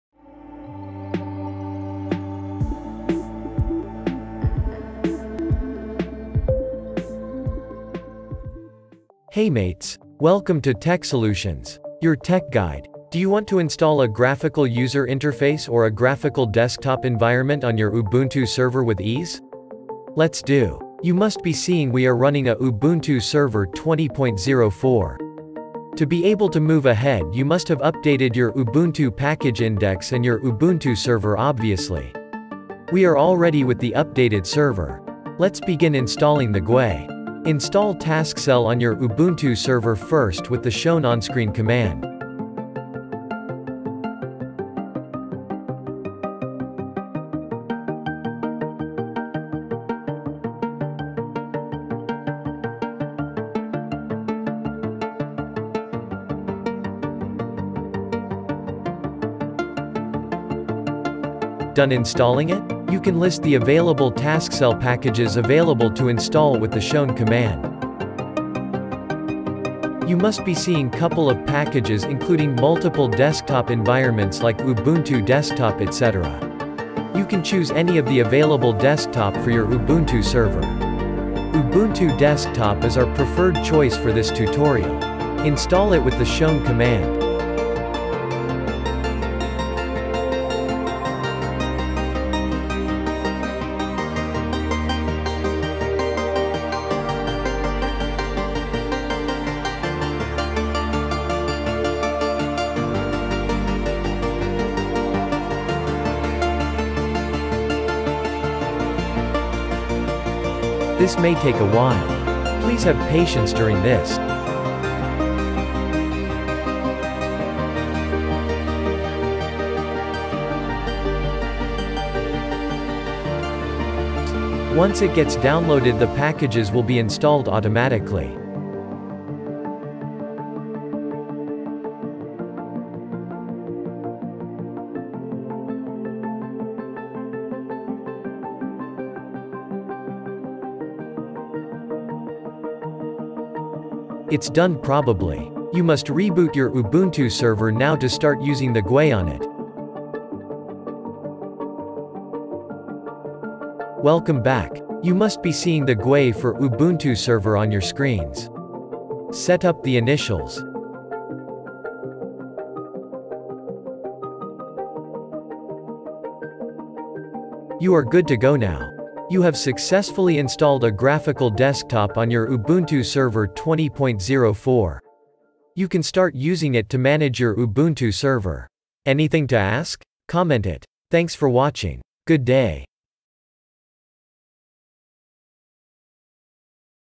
How to Install Desktop / GUI on Ubuntu Server 20.04 GUI Install | Ubuntu Server Desktop 20.04.4 LTS
How to Install Desktop / GUI on Ubuntu Server 20.04 GUI Install | Ubuntu Server Desktop 20.04.4 LTS | By default, Ubuntu Server does not include a Graphical User Interface (GUI). A GUI takes up system resources (memory and processor) that are used for server-oriented tasks. However, certain tasks and applications are more manageable and work better in a GUI environment.